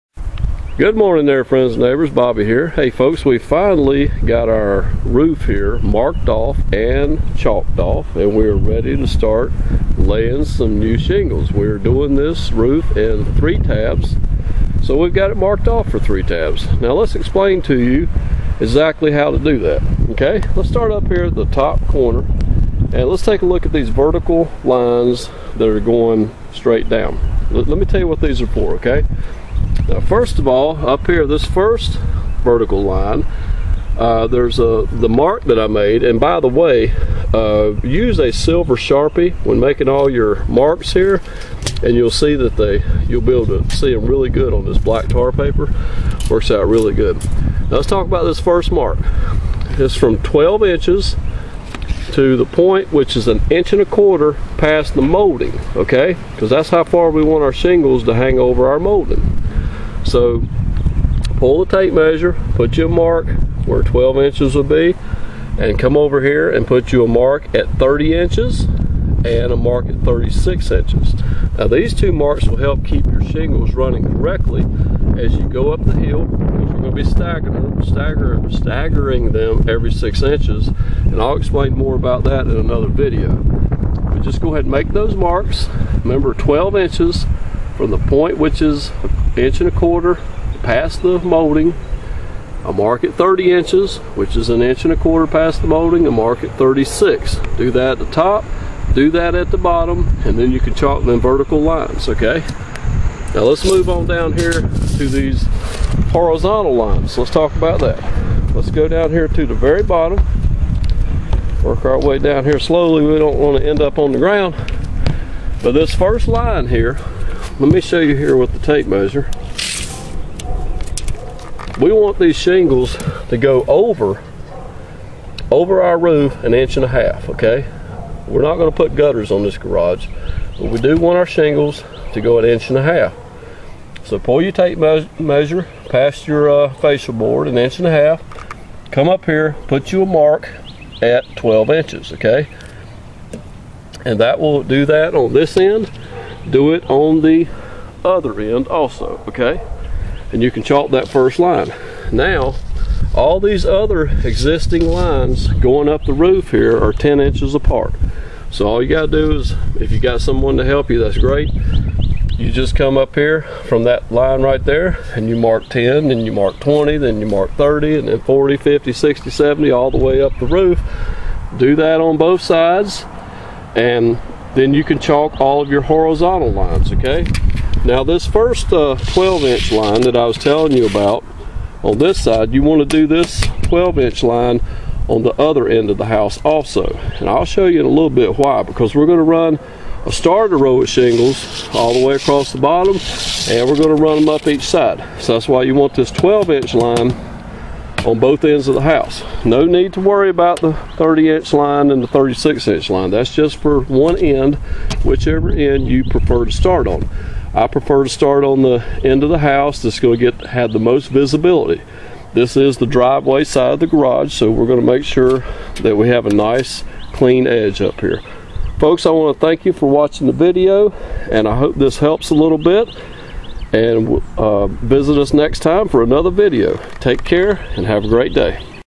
How to : Roofing - Marking off and chalking a (three) 3 tab roof
TOOLS You Will NEED for Roofing from Amazon
Bostitch Coil Nailer
Shingle Remover
Roof Snake
Tool Belt

This video touches base on the importance of marking off and chalking the roof before you start laying your three (3) tab shingles.  Have a great day!

for as little as $1 per month